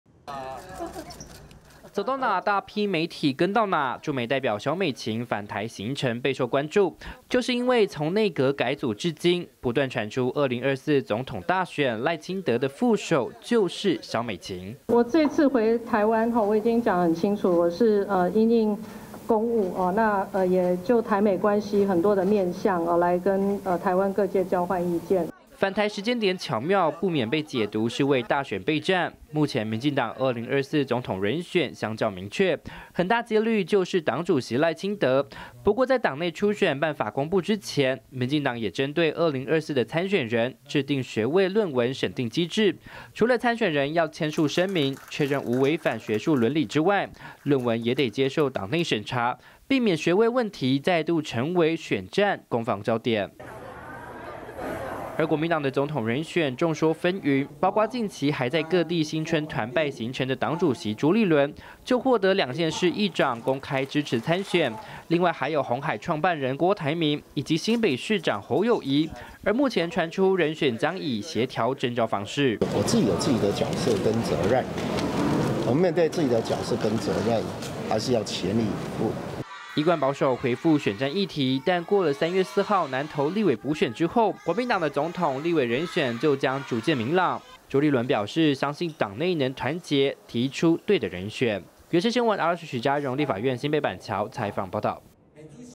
蕭美琴返台述職 被解讀將搭檔賴清德參選｜每日熱點新聞｜原住民族電視台
駐美代表蕭美琴此次返台是為了因應公務，與各界交換台美關係的情況。不過時間點巧妙，也被解讀是為2024總統大選布局。